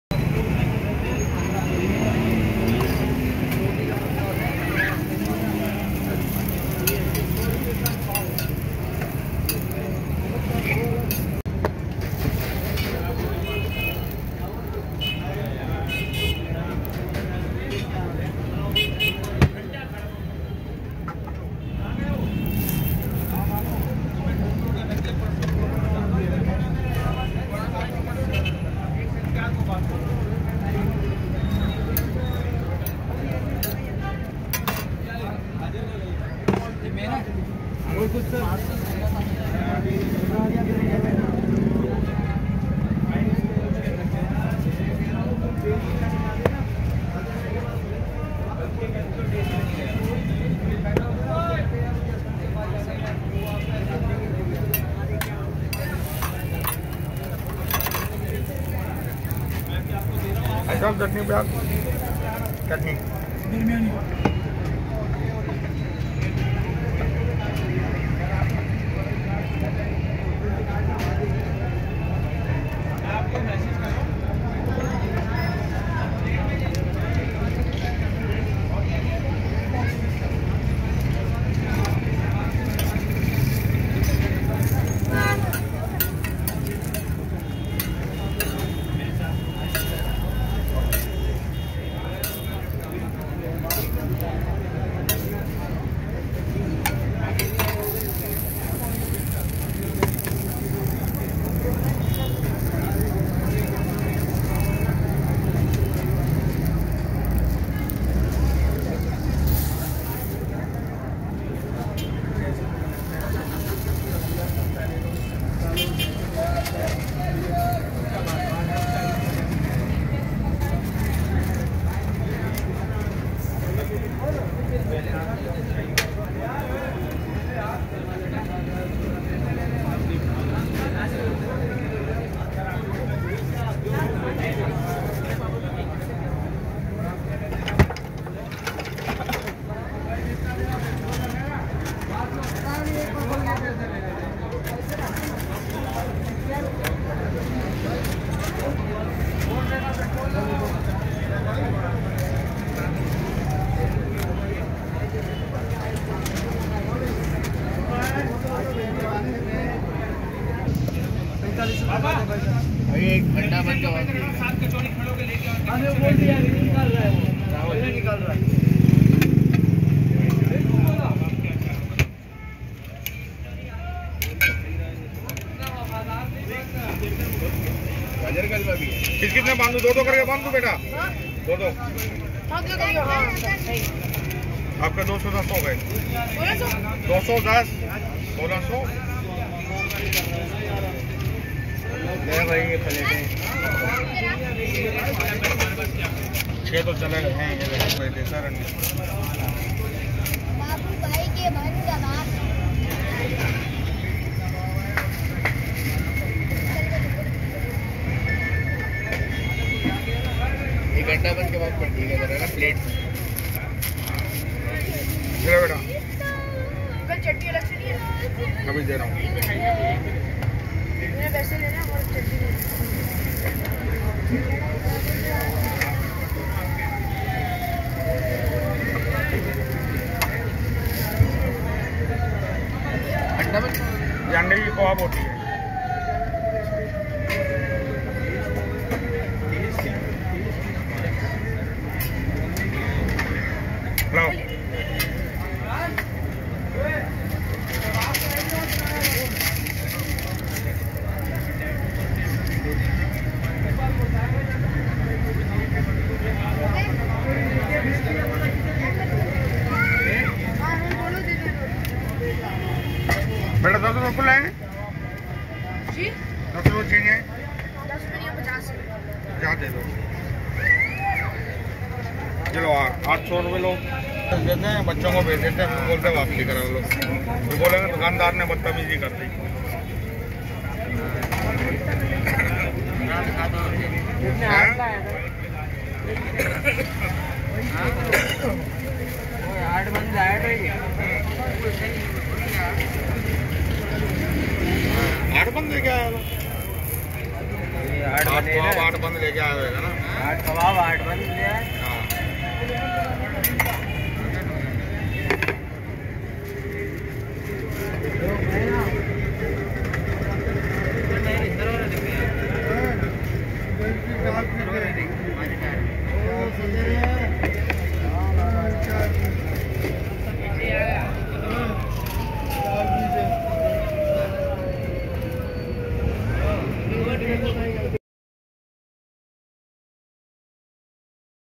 FAMOUS BABU BHAI BUN KABAB STREET FOOD IN BURNS ROAD | STREET FOOD IN KARACHI PAKISTAN
Babu Bhai Bun Kabab One of The Best Bun Kabab Street Food in Burns Road Karachi Pakistan.

Shop Name Babu Bhai Bun Kabab

Address Burns Road Karachi Pakistan

Price Bun Kabab RS 70